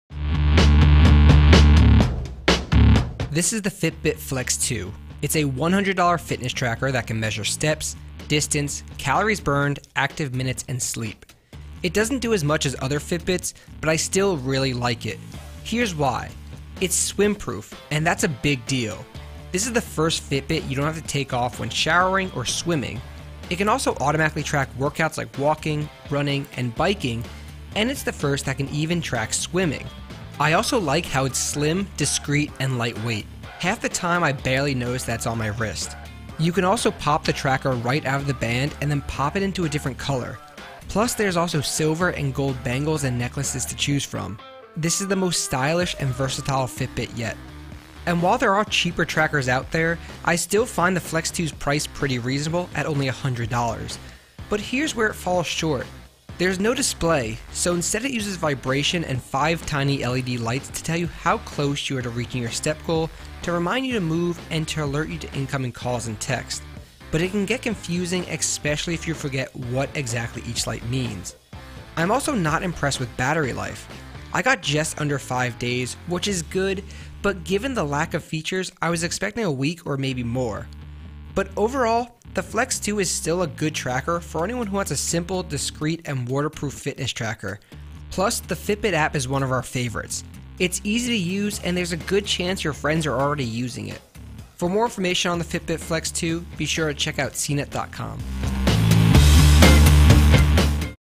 The swim-proof Fitbit Flex 2 is a winner
Looking for a discreet and stylish fitness tracker? Check out the Fitbit Flex 2.